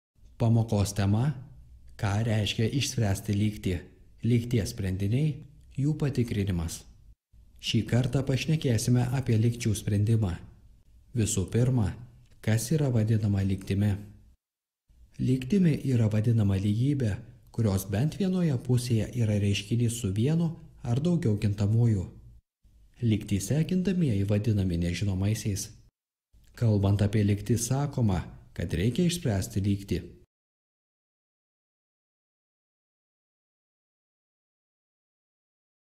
Pirmojo laipsnio lygties sprendimo algoritmas - Mokslinčius.lt
Šioje pamokoje pakalbėsime apie pirmojo laipsnio, kaip vadinamų tiesinių, lygčių sprendimą. Iš pradžių prisiminsime, ką vadiname pirmojo laipsnio lygtimis, o toliau pereisime prie pavyzdžių nagrinėjimo. Išsiaiškinsime, kokie yra skirtingų pirmojo laipsnio lygčių sprendimo žingsniai bei kada sprendiniai gali būti visi realieji skaičiai arba sprendinių gali nebūti iš viso.